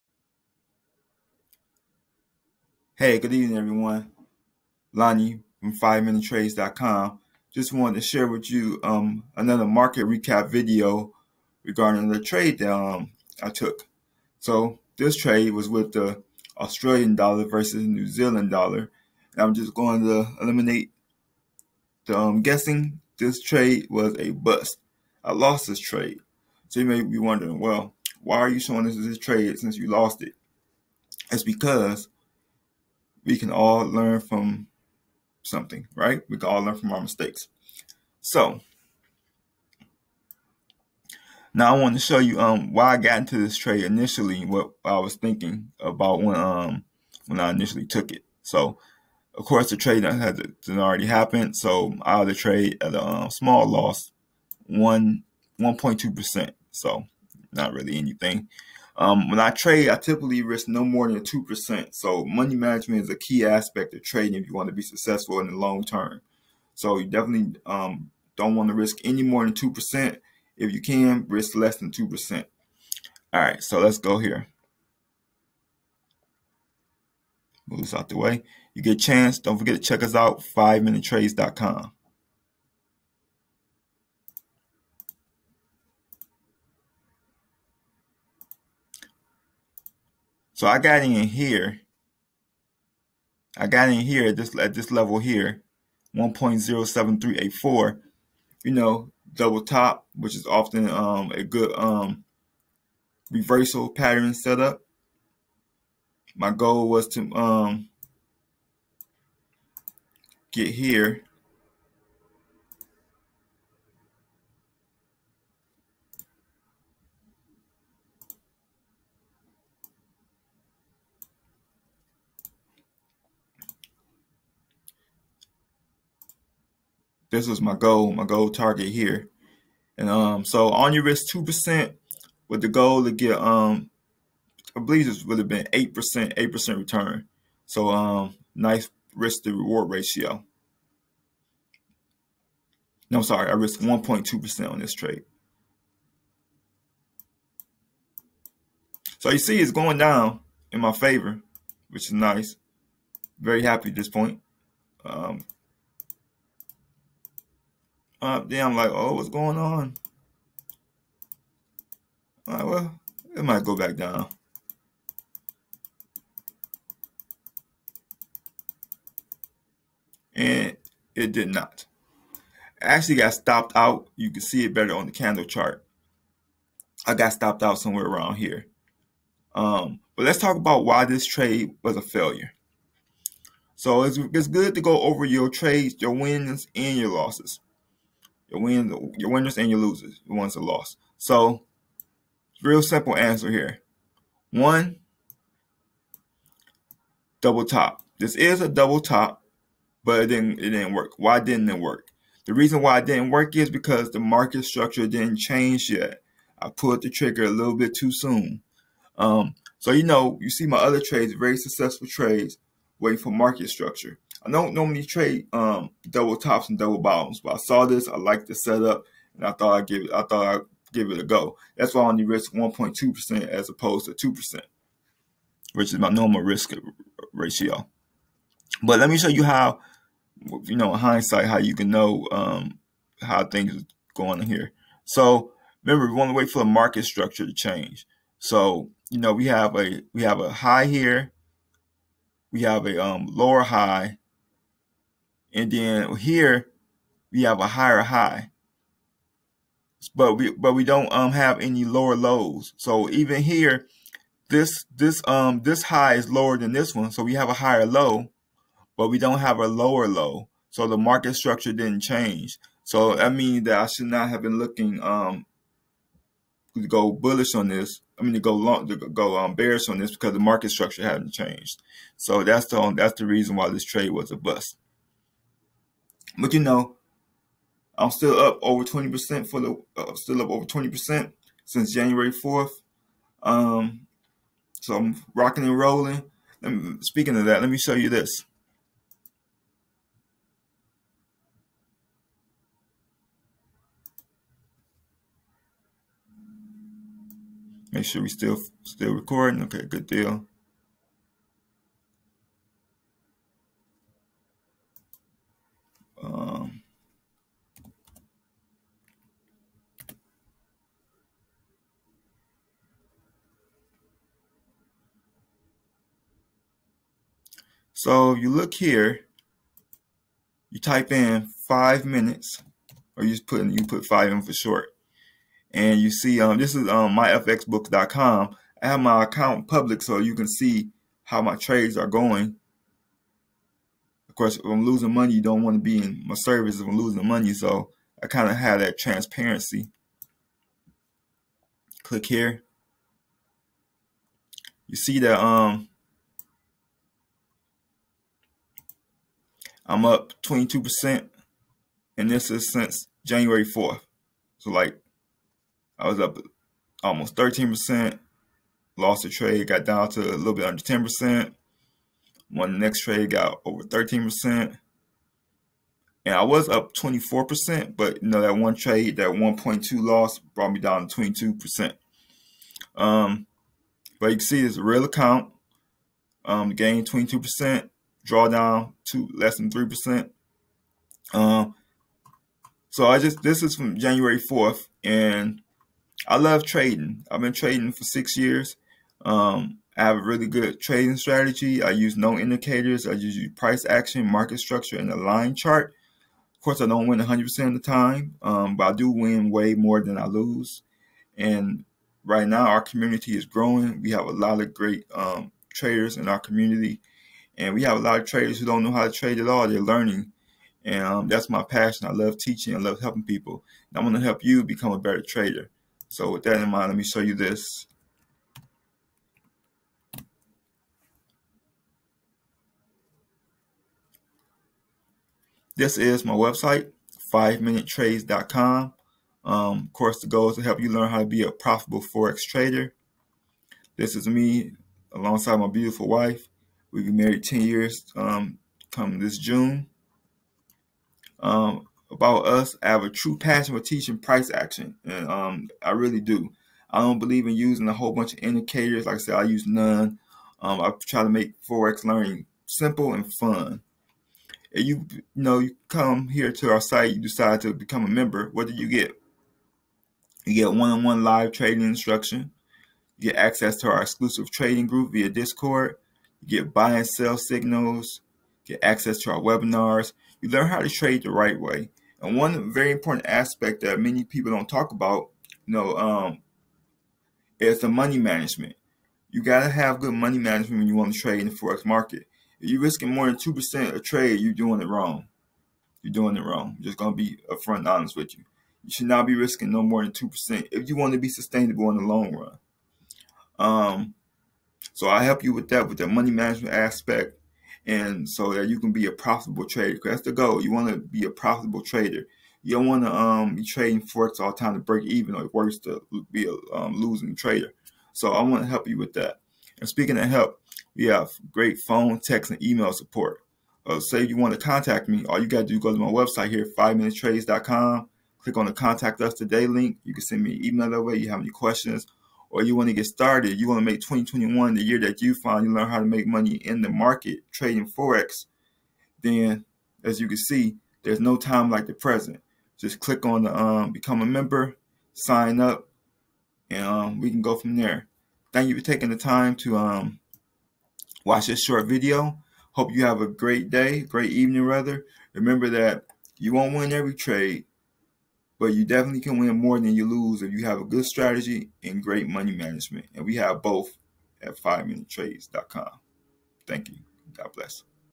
AUDNZD Trading Recap 1/11/2021
Greetings fellow traders! I wanted to share with you a trade that I lost and why. I hope that you find the info useful.